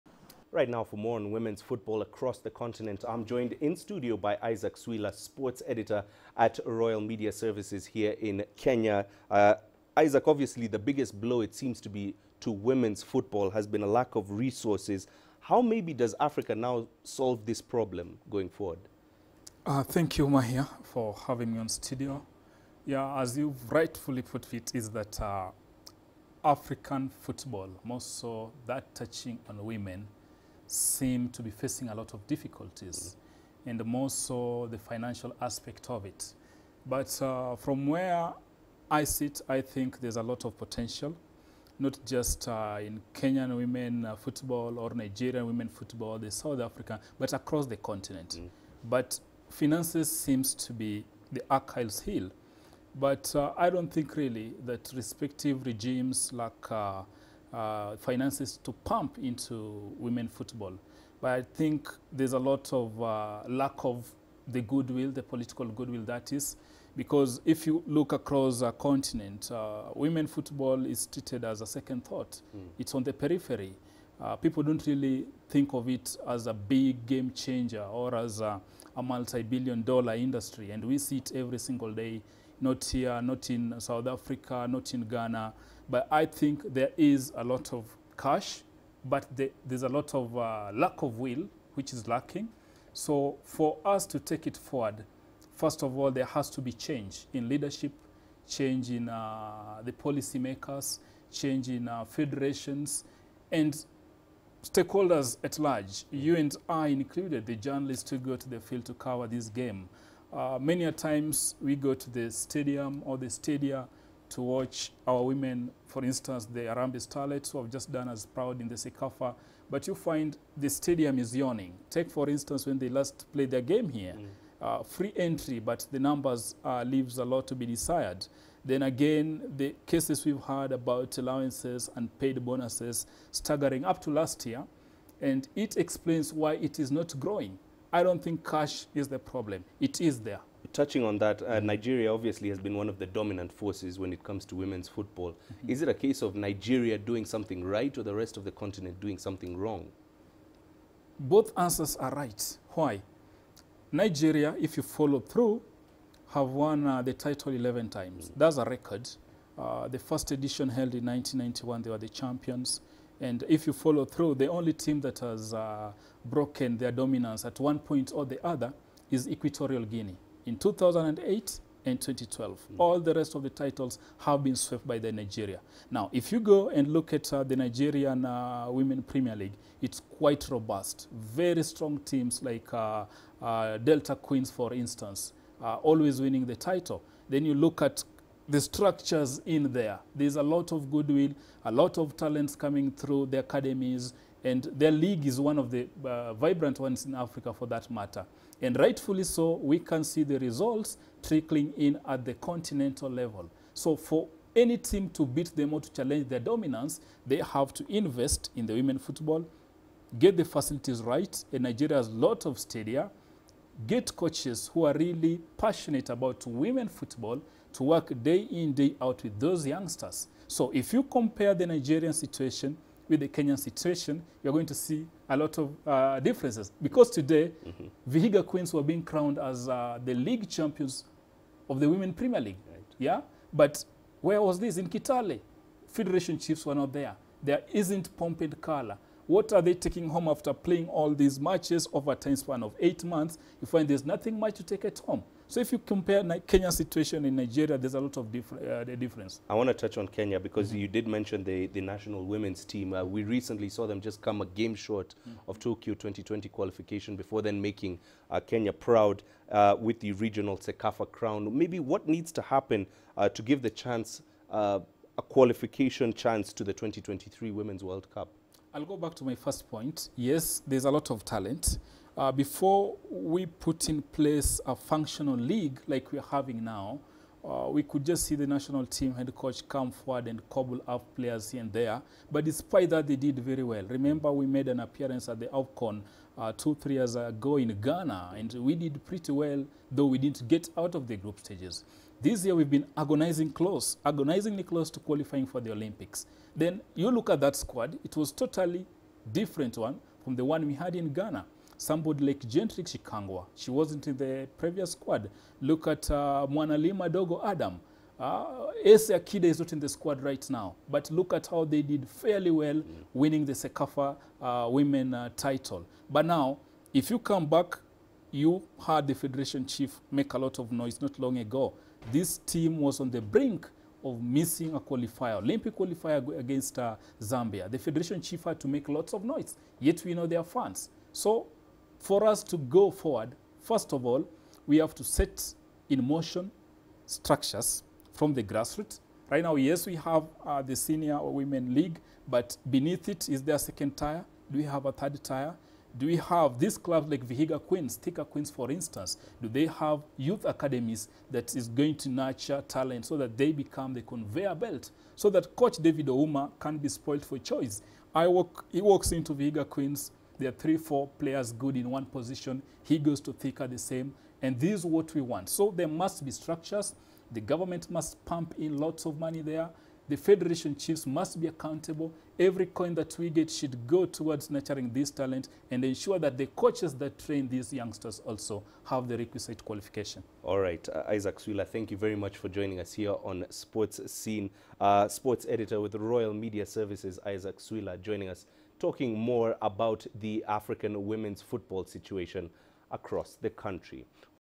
State of women's football in Africa
Women's football in Africa has had mixed fortunes. While some federations enjoy worthwhile success, others are struggling to keep the sport alive in their countries. But what is the true state of women's football in the continent?
For this discussion, Mahia Mutua sought out Isaac Swila, a Sports Editor, for his insights.